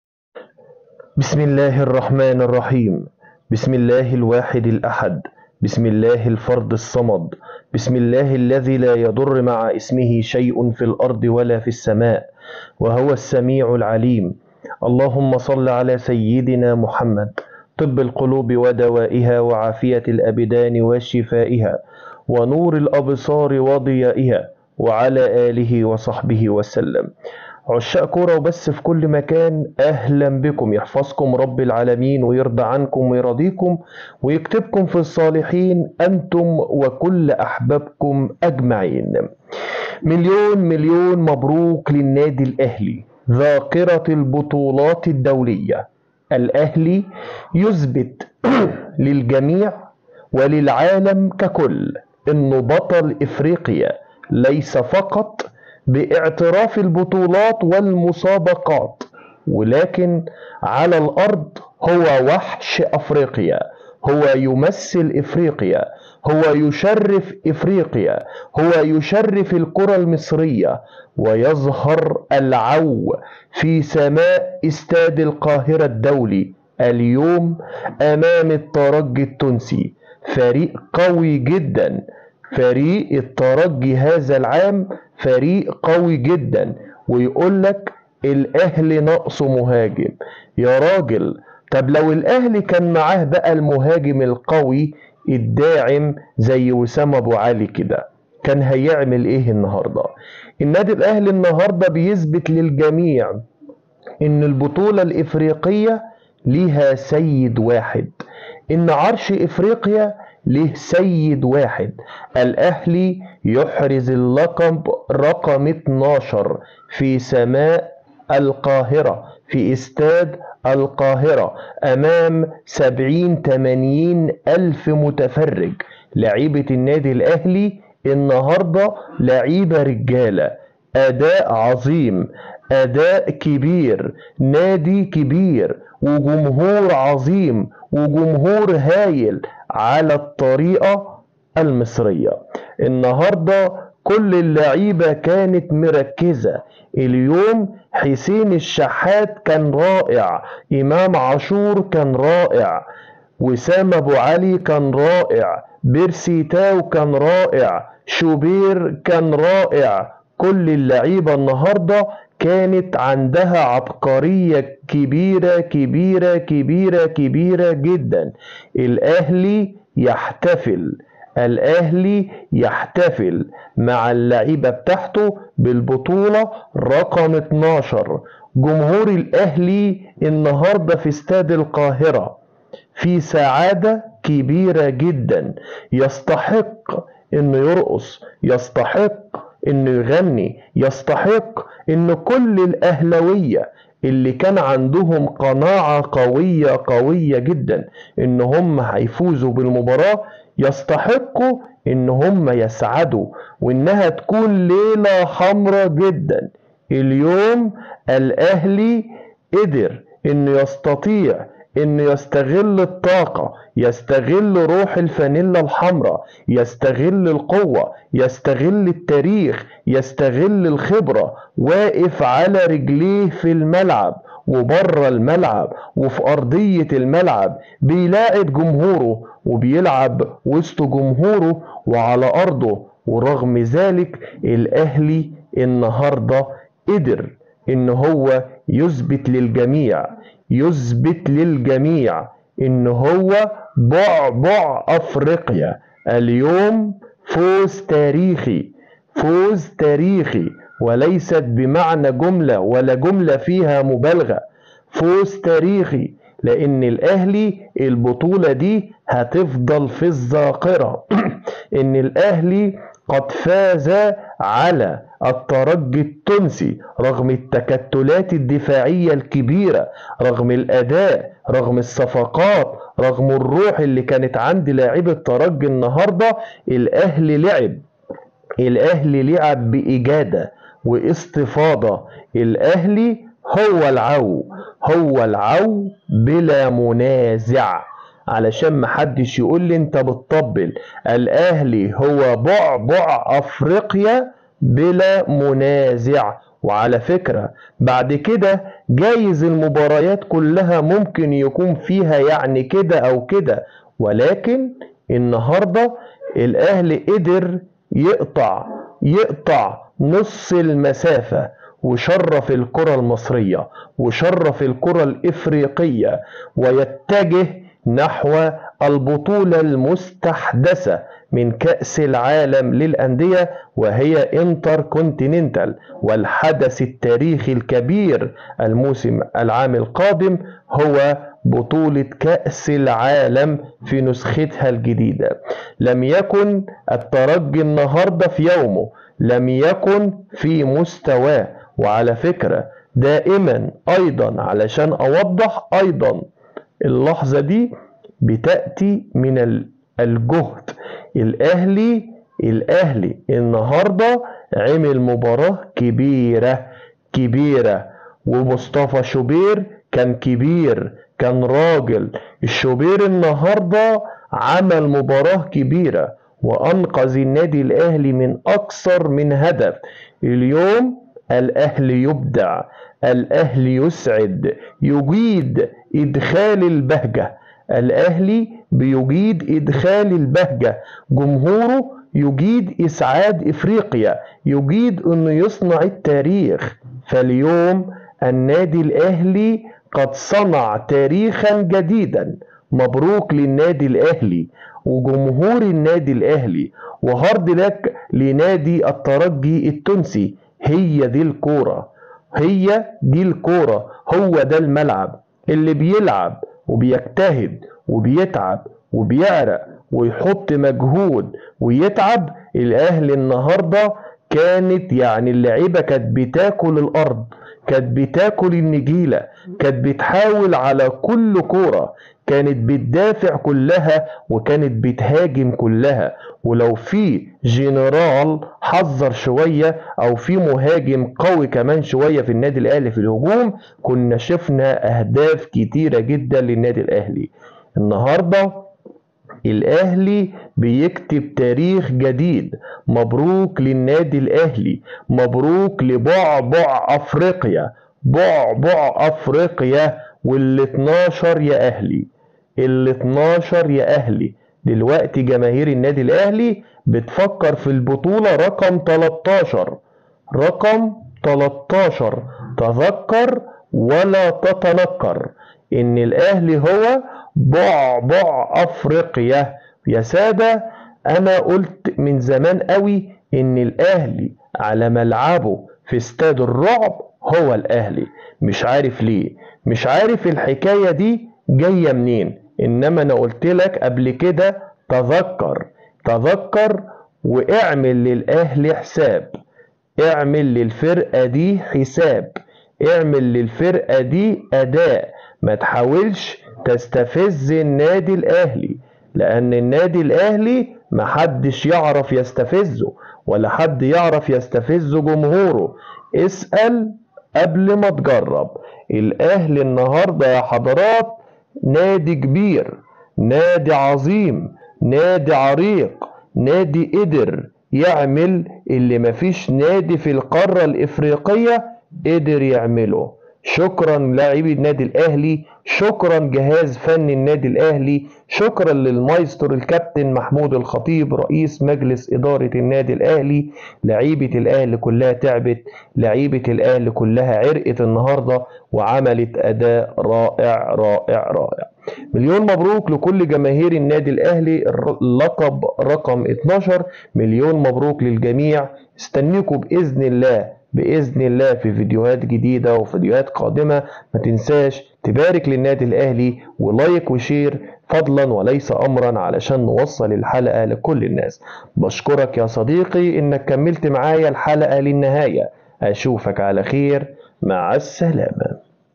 مقدمة للتاريخ المنيسي:بعد فوز الاهلي على الترجي وتتويج بدوري ابطال افريقيا اذا حضر الملك غاب الجميع😱
مقدمة للتاريخ المنيسي:بعد فوز الاهلي على الترجي وتتويج بدوري ابطال افريقيا اذا حضر الملك غاب الجميع😱للمذيد لا تنسي الإشتراك بالقناة وتفعيل الجرس 🔔وشكرا لكم